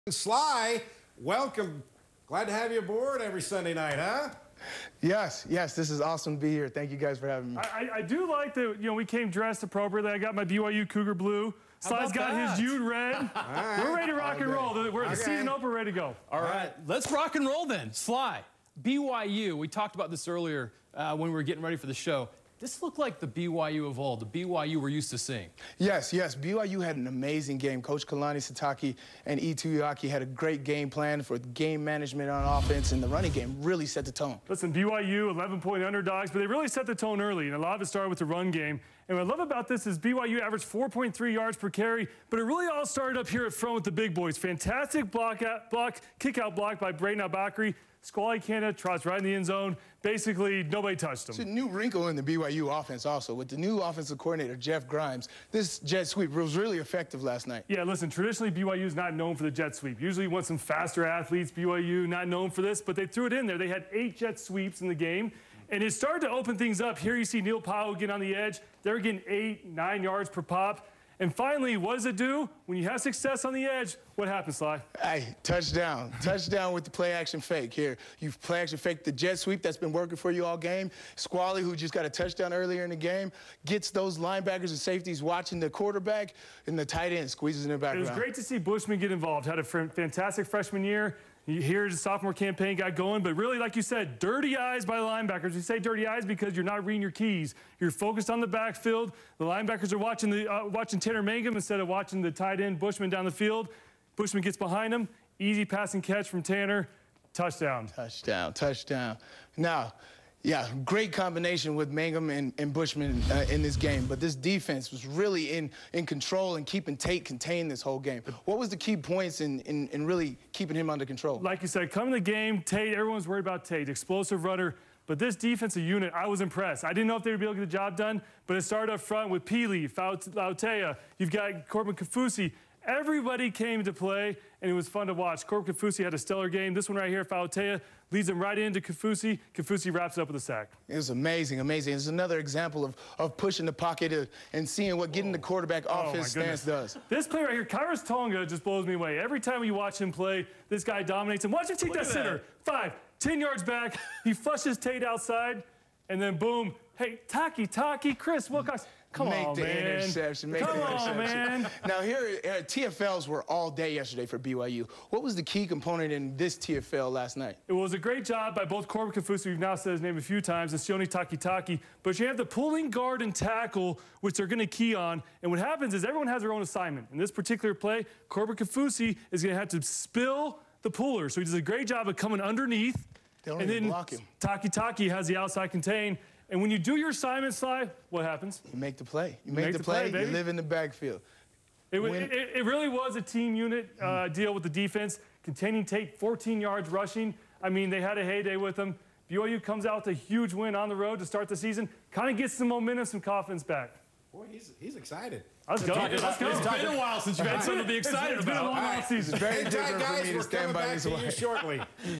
College Football Breakdown: BYU Cougars vs Arizona Wildcats
David Nixon and Stevenson Sylvester are in for the first week of the college football season breaking down BYU's 28-23 win against Arizona.